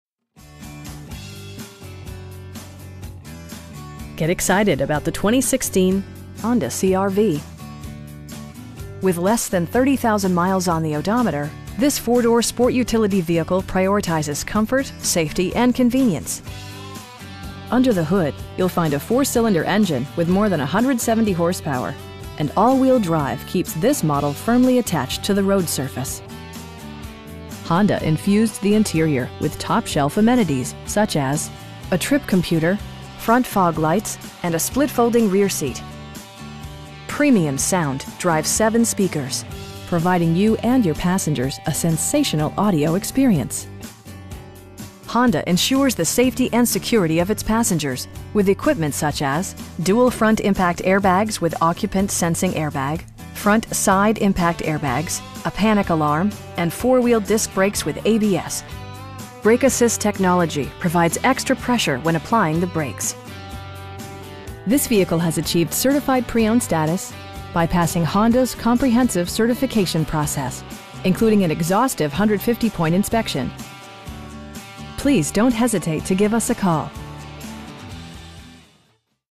2016 Honda CR-V EX-L in Longmont, CO 80501
Frontier Honda Ltd. - Honda Dealer CO
710 S Main Street
in Longmont, CO 80501

Get excited about the 2016 Honda CR-V. With less than 30,000 miles on the odometer, this 4 door sport utility vehicle prioritizes comfort, safety and convenience. Under the hood you'll find a 4 cylinder engine with more than 170 horsepower, and all wheel drive keeps this model firmly attached to the road surface. Honda infused the interior with top shelf amenities, such as: variably intermittent wipers, a trip computer, heated seats, front fog lights, power windows, an overhead console, and a split folding rear seat. Features such as automatic climate control and leather upholstery prove that economical transportation does not need to be sparsely equipped. For drivers who enjoy the natural environment, a power moon roof allows an infusion of fresh air. Premium sound drives 7 speakers, providing you and your passengers a sensational audio experience. Honda ensures the safety and security of its passengers with equipment such as: dual front impact airbags with occupant sensing airbag, front side impact airbags, traction control, a panic alarm, and 4 wheel disc brakes with ABS. Brake assist technology provides extra pressure when applying the brakes. This vehicle has achieved Certified Pre-Owned status, by passing Honda's comprehensive certification process, including an exhaustive 150-point inspection! We pride ourselves in consistently exceeding our customer's expectations. Please don't hesitate to give us a call.